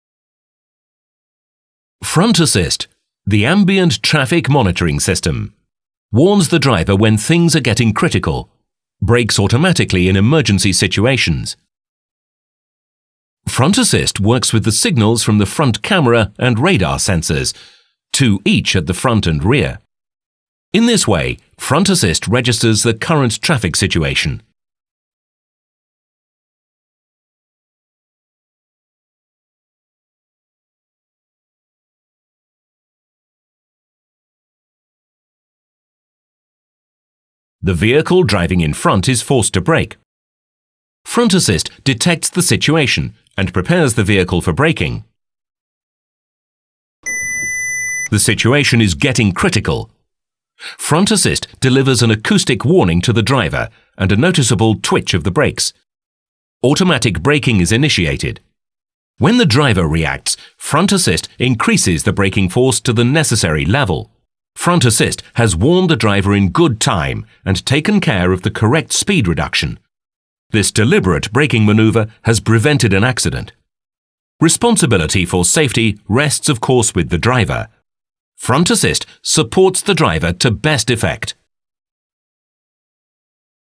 Front Assist | Tecnologia | Volkswagen
A Volkswagen investe em utensílios de alta tecnologia para satisfazer plenamente todas necessidades. Confira

Front Assist, o sistema de monitoramento de tráfego. Ele avisa o motorista quando a situação está ficando crítica, freando automaticamente em situações de emergência.